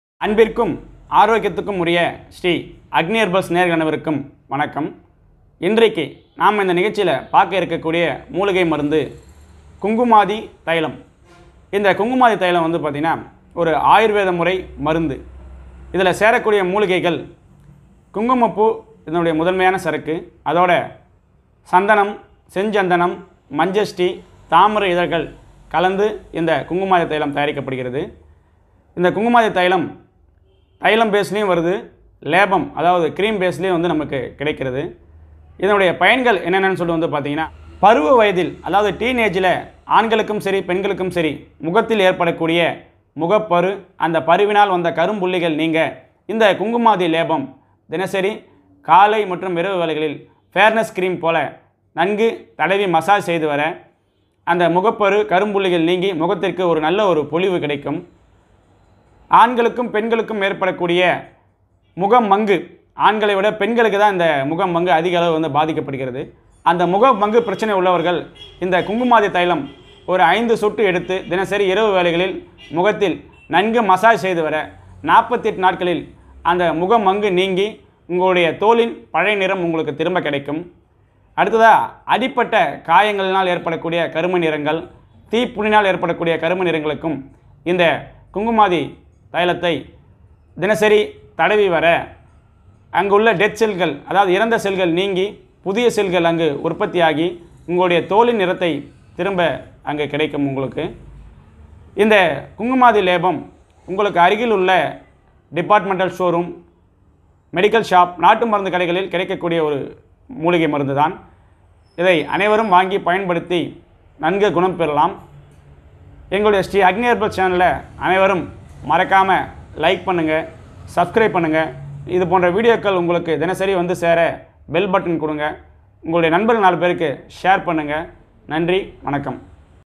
Kunkumadi Thailam - குங்குமாதி தைலம்
பயன்கள் : முகப்பரு, கரும்புள்ளிகள் ,முக மங்கு, அடிப்பட்ட காயத்தினால் ஏற்பட்ட கருமை நிறத் திட்டுகள், தீக்காயத்தால் ஏற்பட்ட கருமை நிறம் நீங்க சிறந்த மருந்து.